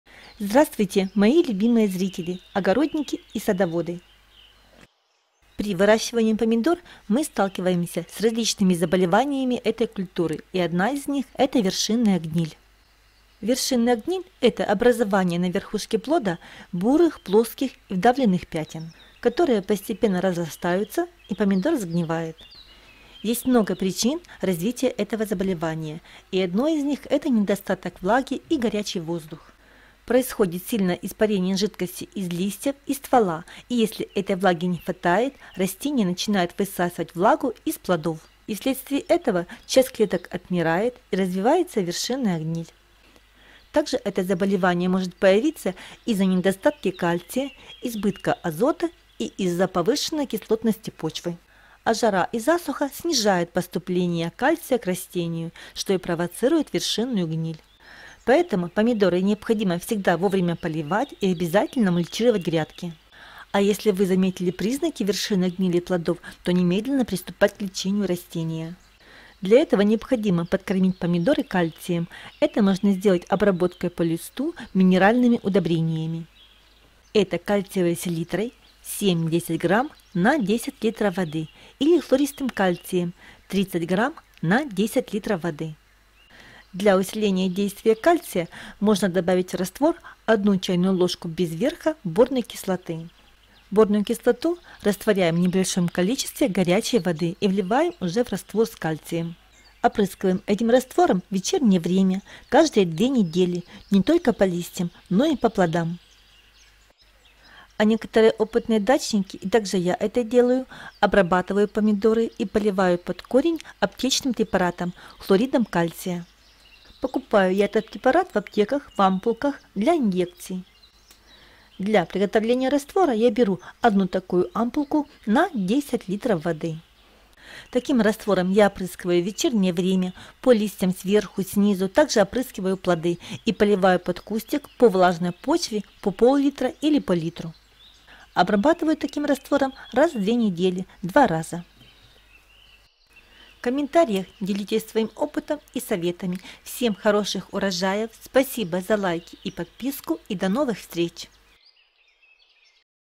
ВСЕГО 1 АМПУЛА СПАСЕТ ТОМАТЫ ОТ ЗАГНИВАНИЯ.БЫСТРО, СОВЕРШЕННО БЕЗОПАСНО И ВЫ С УРОЖАЕМ!ВЕРШИННАЯ...
ЧЕМ ПОДКОРМИТЬ ТОМАТЫ ОТ ВЕРШИННОЙ ГНИЛИ.
ВСЕГО 1 АМПУЛА ХЛОРИДА КАЛЬЦИЯ СПАСЕТ ТОМАТЫ ОТ ЗАГНИВАНИЯ,ОТ ВЕРШИННОЙ ГНИЛИ .БЫСТРО, СОВЕРШЕННО БЕЗОПАСНО И ВЫ С УРОЖАЕМ!ВЕРШИННАЯ...